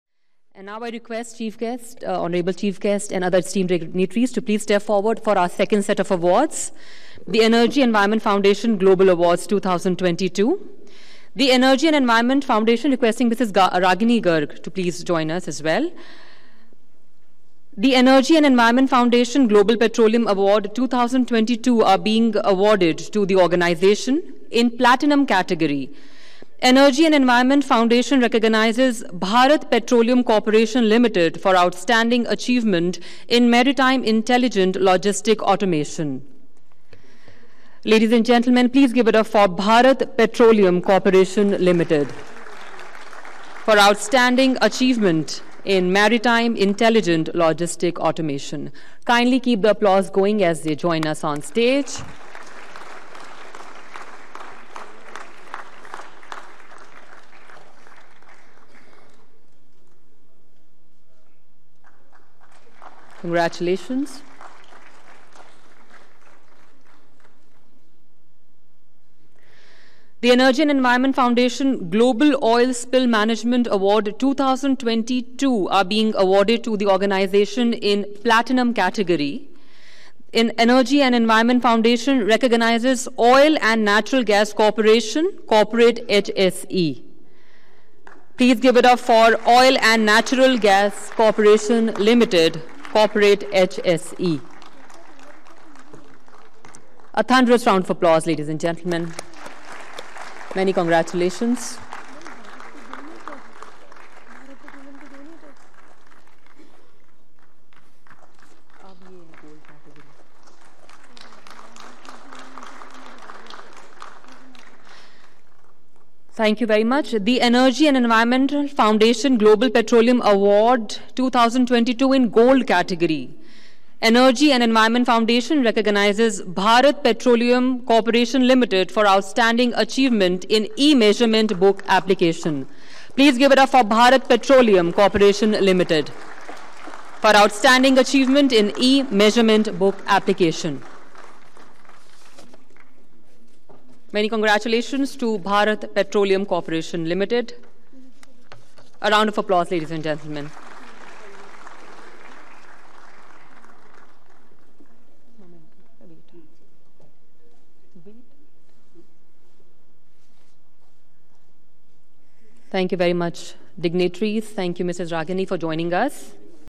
The Energy and Environment Foundation Global Awards 2022 | WorldPetroTech & WorldOilSpill
The Energy and Environment Foundation Global Awards 2022 (Petroleum / Oil Spill Management Sector) the World Petroleum Technology Congress-2022 & World Oil Spill Conference & Expo-2022 from 7-8 December 2022 at Vigyan Bhawan, New Delhi, India